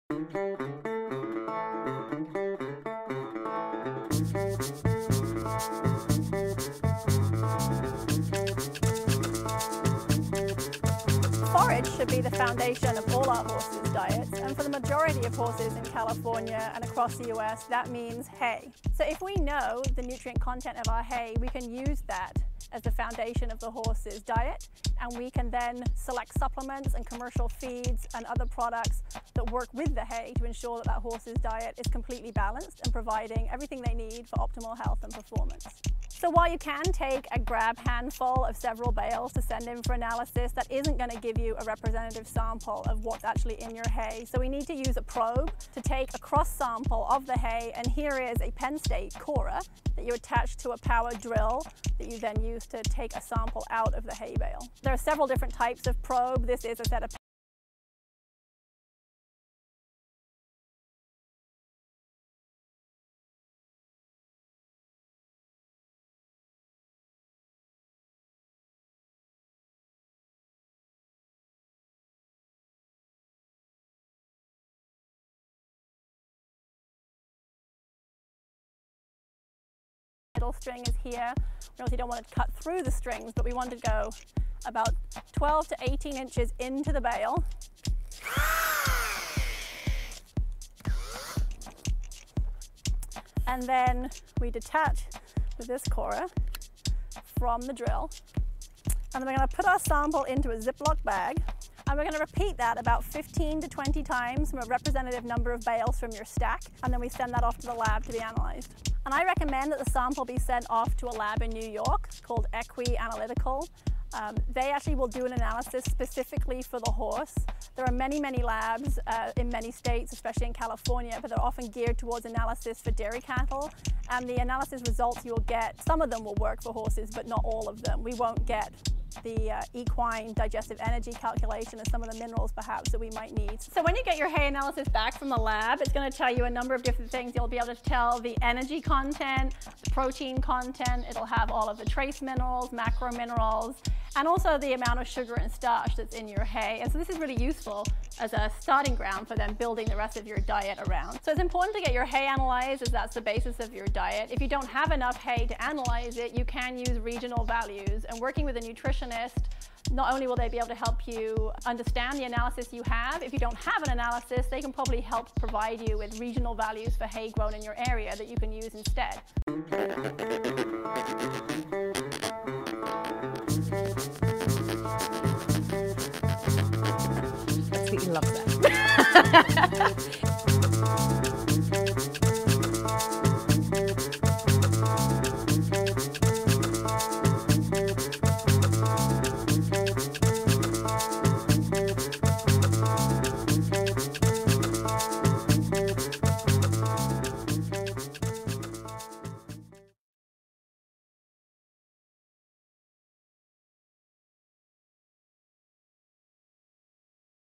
How To Take A Hay Sample For Analysis
t's not uncommon for horse owners to select hay simply based on what looks "good", or the recommendations of others, or what's available at the local feed store. How can you learn what's really in that hay? Is it going to provide proper nutrition for your horse's needs? Clair Thunes, PhD, shows you how to take a hay sample to send to a lab for nutritional analysis. In addition to being an independent equine nutritionist, Dr. Thunes is a certified hay sampler with the National Forage Hay Testing Association. In her Summit Equine Nutrition program, she has worked with a wide range of horses from lactating mares to competitive driving horses, and with a variety of physiological problems including insulin resistance and muscle myopathies. Dr. Thunes believes in finding the right balance in the horse's diet for optimal performance.